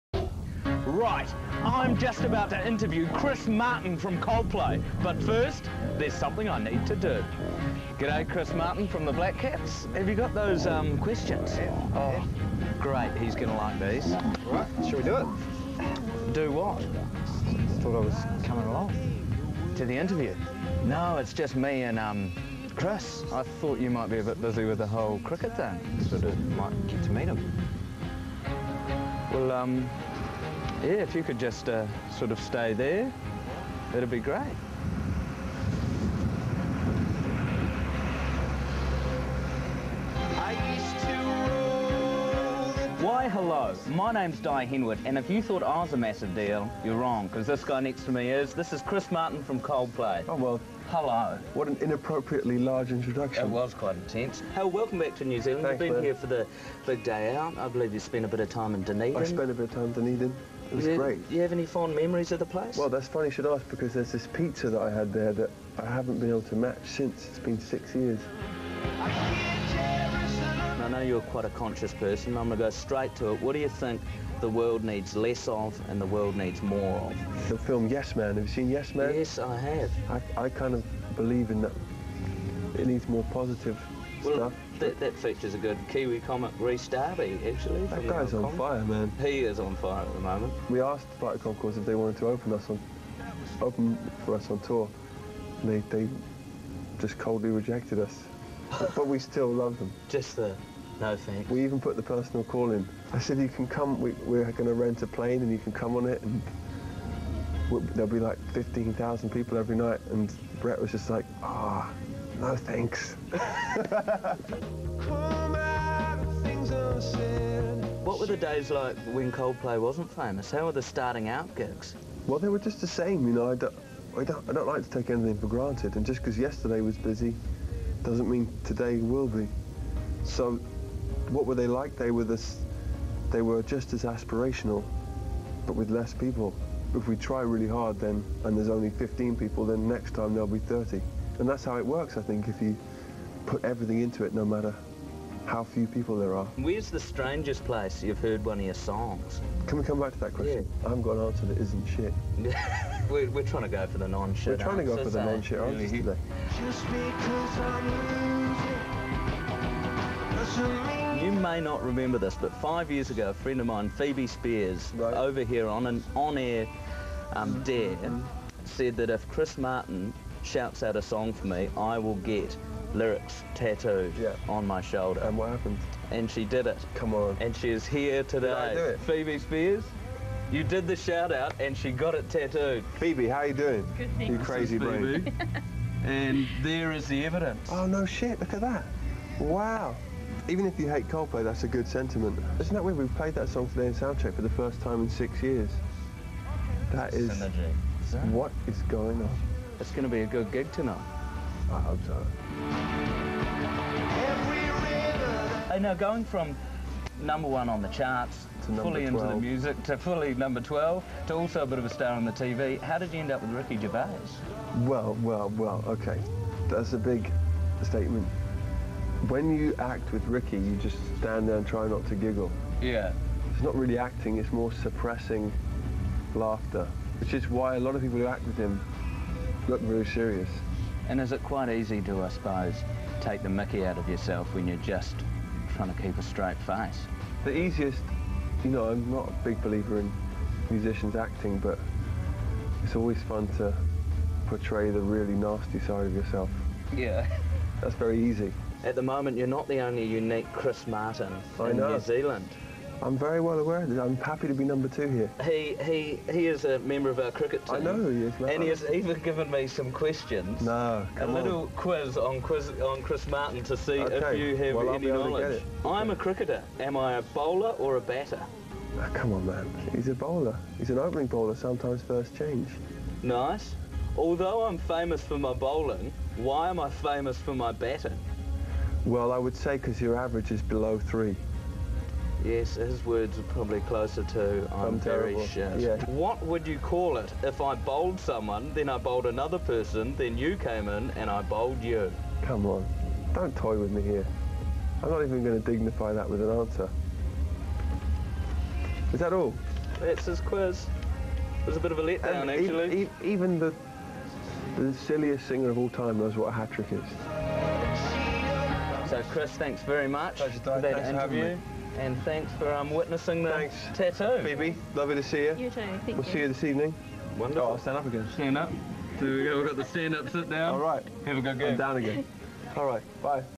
Chris Martin (Coldplay) interview 2009
Dai Henwood interviews Chris Martin from Coldplay, during their Viva La Vida tour in New Zealand, 2009.